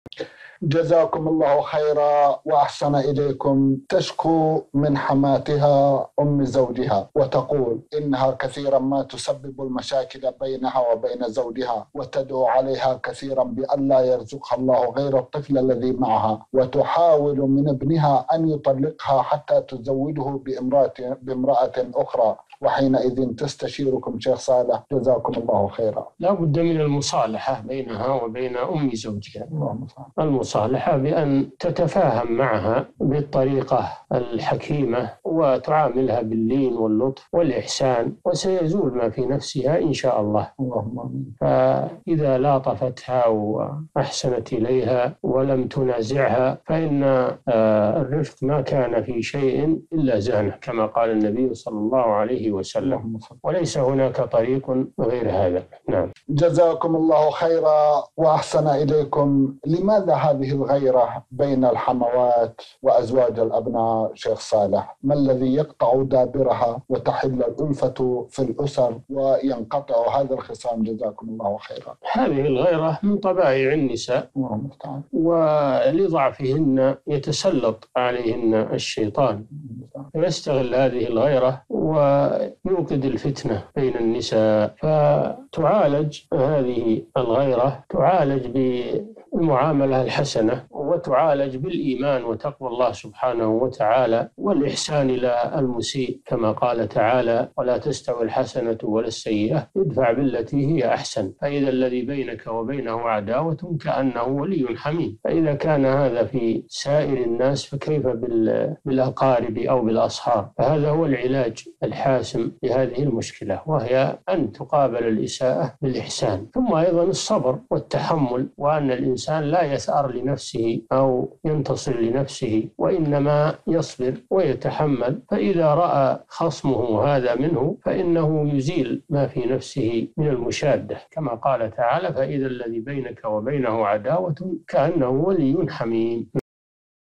4917- ملاطفة الزوجة لأم زوجها وترك منازعتها وسد مداخل الشيطان بينهما - الشيخ صالح الفوزان
"السؤال: جزاكم الله خيرًا وأحسن إليكم، تشكو من حماتها أم زوجها، وتقول: إنَّها كثيرًا ما تسبب المشاكل بينها وبين زوجها، وتدعو عليها كثيرًا بألا يرزقها الله غير الطفل الذي معها، وتحاول من ابنها أن يطلقها حتى تزوجه بامرأة أخرى، وحينئذ تستشيركم يا شيخ صالح جزاكم الله خيرًا.
الجواب: لا بد من المصالحة بينها وبين أم زوجها، المصالحة بأن تتفاهم معها بالطريقة الحكيمة، وتعاملها باللين واللطف والإحسان، وسيزول ما في نفسها إن شاء الله، فإذا لاطفتها وأحسنت إليها ولم تنازعها فإن الرفق ما كان في شيء إلا زانه، كما قال النبي ﷺ، وليس هناك طريق غير هذا.
السائل: جزاكم الله خيرًا وأحسن إليكم، لماذا هذه الغيرة بين الحموات وأزواج الأبناء يا شيخ صالح، ما الذي يقطع دابرها وتحل الألفة في الأسر وينقطع هذا الخصام؟ جزاكم الله خيرًا.
الشيخ: هذه الغيرة من طبائع النساء، ولضعفهن يتسلط عليهن الشيطان، ويستغل هذه الغيرة ويوقد الفتنة بين النساء، فتعالج هذه الغيرة تعالج بالمعاملة الحسنة، وتعالج بالإيمان وتقوى الله سبحانه وتعالى والإحسان إلى المسيء، كما قال تعالى ﴿وَلَا تَسْتَوِي الْحَسَنَةُ وَلَا السَّيِّئَةُ ادْفَعْ بِالَّتِي هِيَ أَحْسَنُ فَإِذَا الَّذِي بَيْنَكَ وَبَيْنَهُ عَدَاوَةٌ ‌كَأَنَّهُ ‌وَلِيٌّ ‌حَمِيمٌ﴾ [فصلت: 34]، فإذا كان هذا في سائر الناس، فكيف بالأقارب أو بالأصهار؟ فهذا هو العلاج الحاسم لهذه المشكلة، وهي أن تقابل الإساءة بالإحسان، ثم أيضا الصبر والتحمل، وأن الإنسان لا يثأر لنفسه أو ينتصر لنفسه، وإنما يصبر ويتحمل، فإذا رأى خصمه هذا منه فإنه يزيل ما في نفسه من المشادة، كما قال تعالى ﴿فَإِذَا الَّذِي بَيْنَكَ وَبَيْنَهُ عَدَاوَةٌ ‌كَأَنَّهُ ‌وَلِيٌّ ‌حَمِيمٌ﴾.

السيرة الذاتية :
للاشتراك في قناة الشيخ صالح الفوزان

14_ الشيخ محمد أمين الشنقيطي
يتيوب

15"